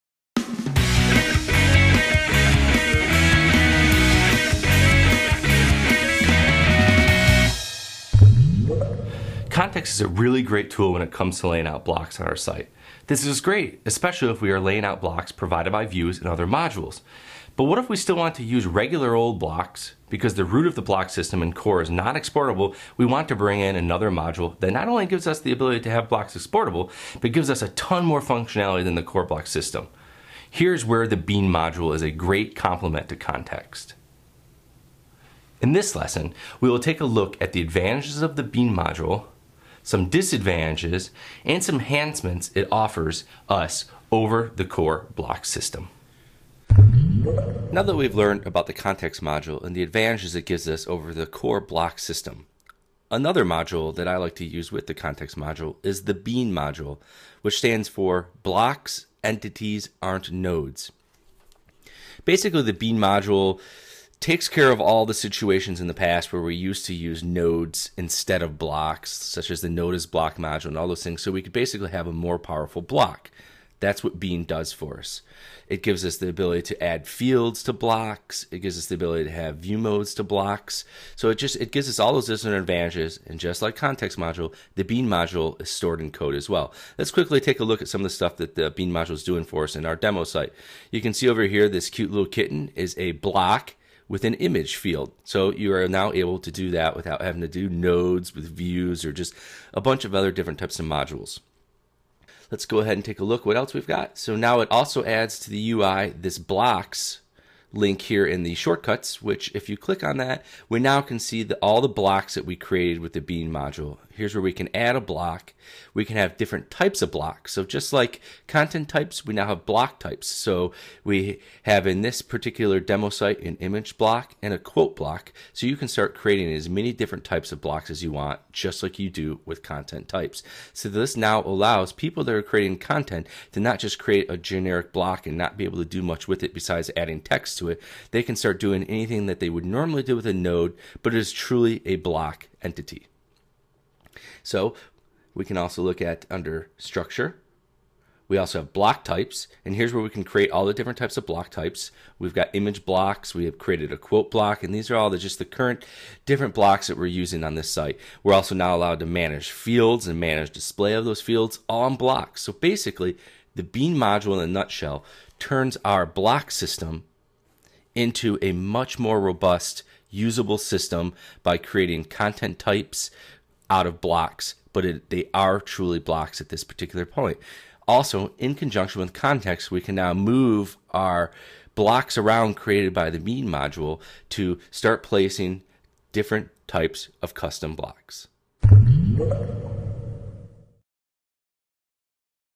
Overview of the Bean Module
Much like using Context to enhance the core block system, the Bean module takes blocks to a whole new level. The Bean module makes blocks act more like content types.  It allows for different block types and for adding fields to blocks. We are also able to manage the display of a block, which comes in handy with Context. We can have a block look different based on the Context of the block. You can watch the rest of this series on Drupalize.Me